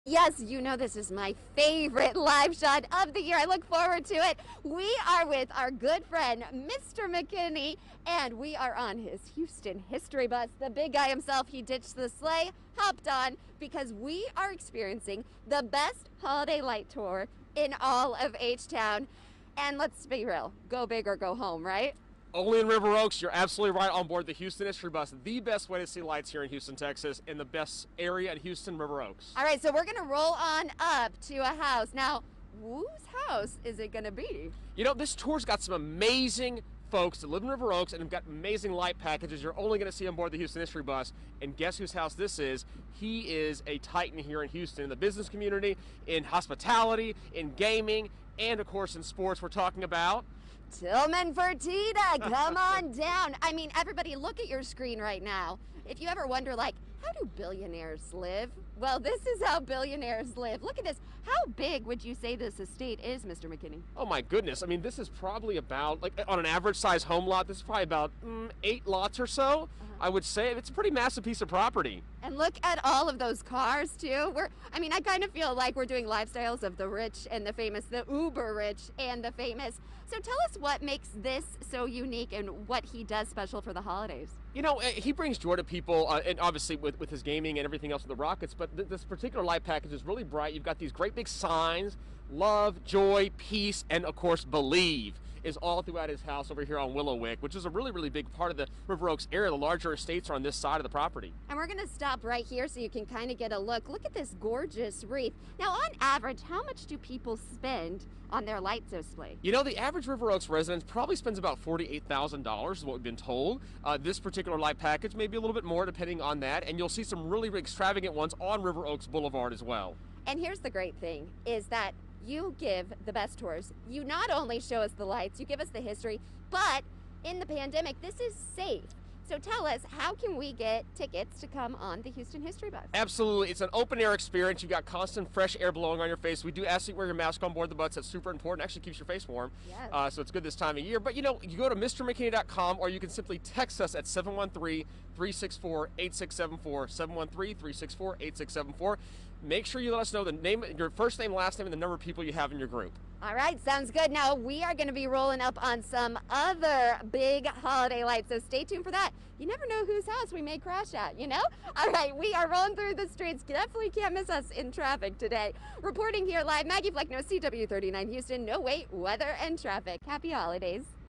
Historic Holiday Lights: Enjoy River Oaks Christmas lights tour on Houston History Bus
Ditch the sleigh and hop on Mister McKinney’s Houston History Bus for his annual River Oaks Holiday Lights Tour. In addition to seeing the beautiful holiday displays, McKinney will teach you the history of the home and homeowners.